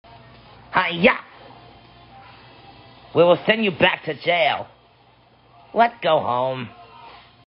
Jay Lines for NGJW Productions
Here you go Dude, I hope you like my jay lines for your legs ninjago stop motion animation.